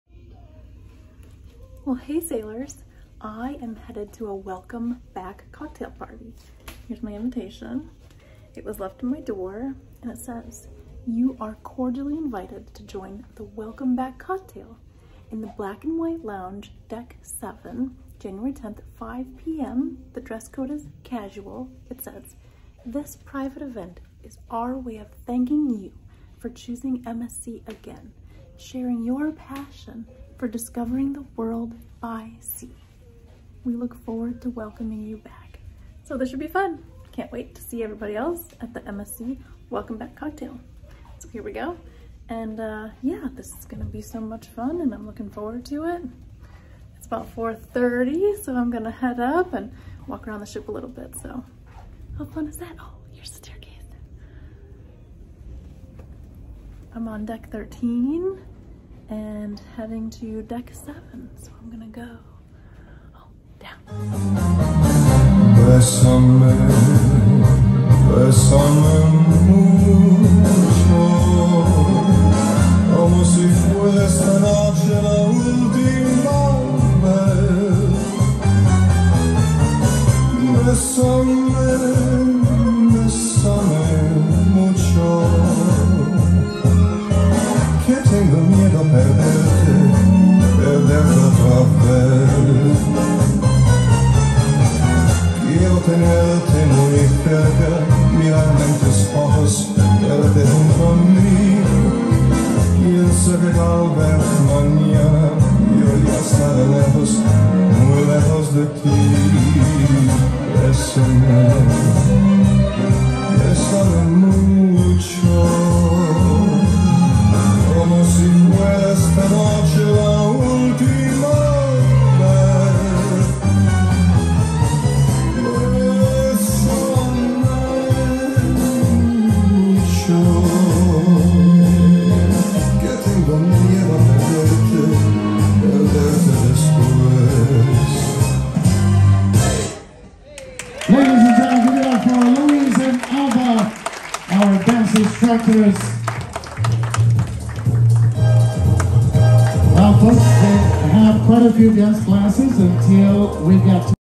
PRIVATE EVENT! Welcome Back Cocktail Party with Dancers!
Follow along as I take you into this PRIVATE EVENT! I was cordially invited by MSC to a private Welcome Back Cocktail Party for returning passengers. I take you inside the lounge, where professional dancers performed a beautiful dance!